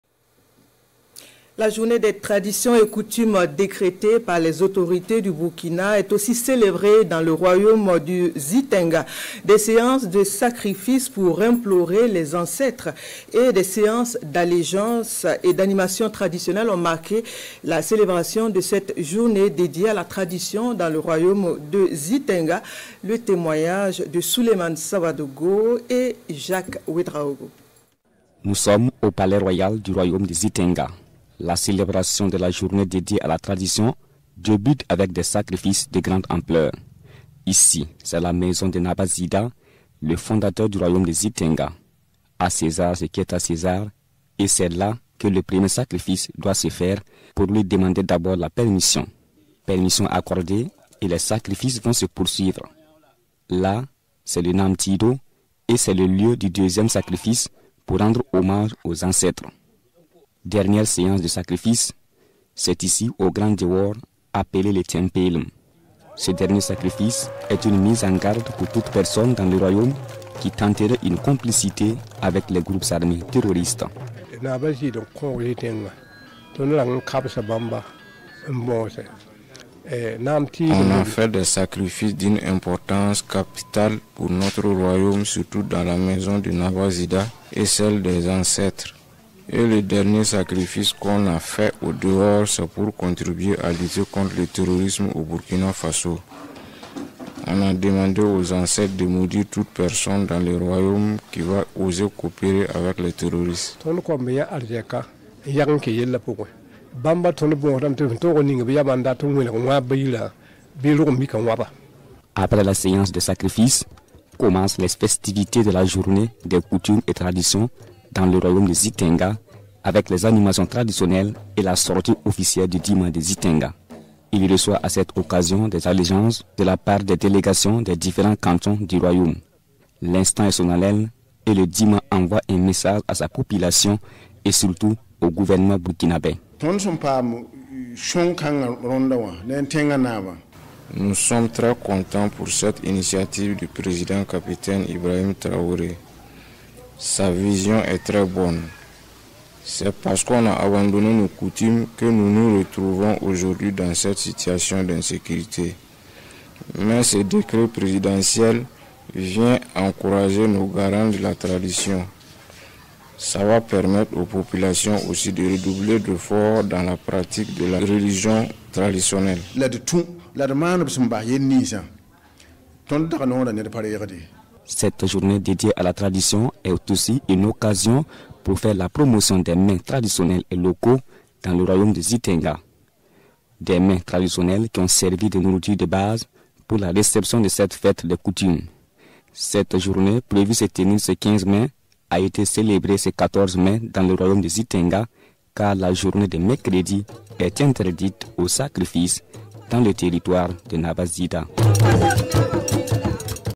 Journée des traditions au royaume de Zitenga , des séances de sacrifices pour implorer les ancêtres
Journée des coutumes et traditions : dans le royaume de Zitenga , des séances de sacrifices pour implorer les ancêtres, et des séances d’allégeances et d’animations traditionnelles ont marqué la célébration du 15 mai